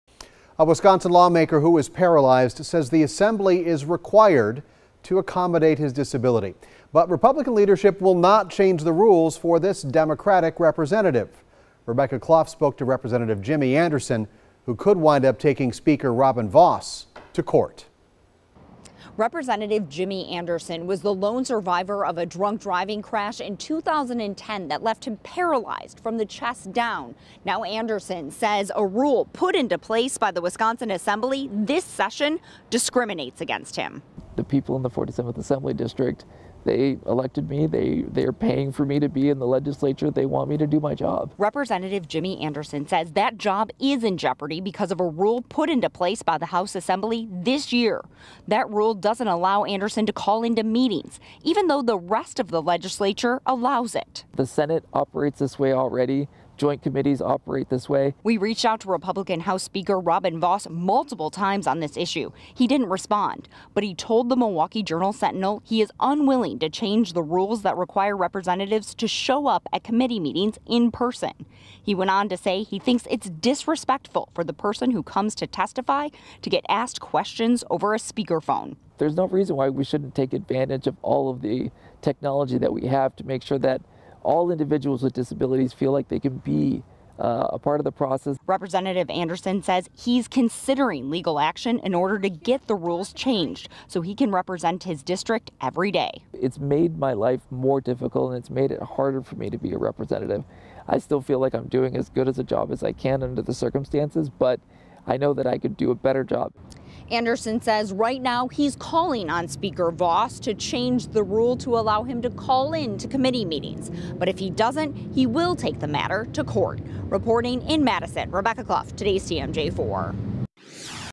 Paralyzed state lawmaker not allowed to call into meetings
A Wisconsin state lawmaker who is paralyzed isn't allowed to call into certain meetings and said he is being discriminated against.